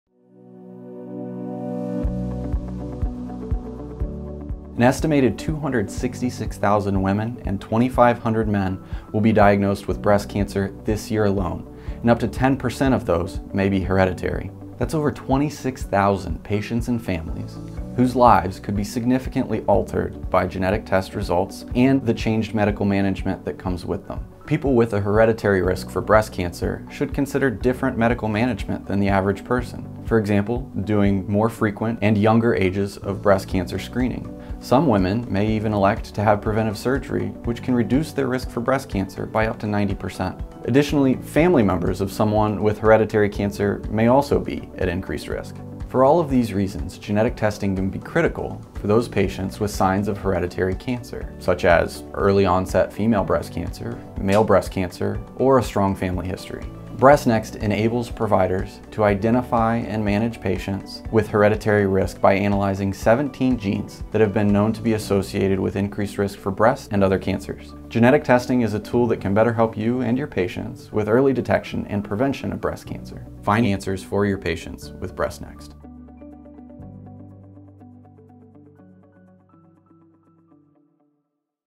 Genetic Testing for Hereditary Breast Cancer: BreastNext® | Ambry Genetics
For more information about our breast cancer genetic testing panel go

Understanding high-risk breast cancer patients such as those with early-onset or multiple diagnoses of breast cancer, male breast cancer, and/or with a family history of disease is made clearer with BreastNext, a 17 gene panel, offering more precision to identify and manage hereditary breast cancer.

Test Description
BreastNext analyzes 17 genes (listed above). All genes are evaluated by next generation sequencing (NGS) or Sanger sequencing of all coding domains, and well into the flanking 5’ and 3’ ends of all the introns and untranslated regions. In addition, sequencing of the promoter region is performed for PTEN (c.-1300 to c.-745). The BRCA2 Portuguese founder mutation, c.156_157insAlu (also known as 384insAlu) is detected by NGS and confirmed by PCR and agarose gel electrophoresis. Clinically significant intronic findings beyond 5 base pairs are always reported. Intronic variants of unknown or unlikely clinical significance are not reported beyond 5 base pairs from the splice junction. Additional Sanger sequencing is performed for any regions missing or with insufficient read depth coverage for reliable heterozygous variant detection. Reportable small insertions and deletions, potentially homozygous variants, variants in regions complicated by pseudogene interference, and single nucleotide variant calls not satisfying 100x depth of coverage and 40% het ratio thresholds are verified by Sanger sequencing. Gross deletion/duplication analysis for the covered exons and untranslated regions of all 17 genes is performed using read-depth from NGS data with confirmatory multiplex ligation-dependent probe amplification (MLPA) and/or targeted chromosomal microarray.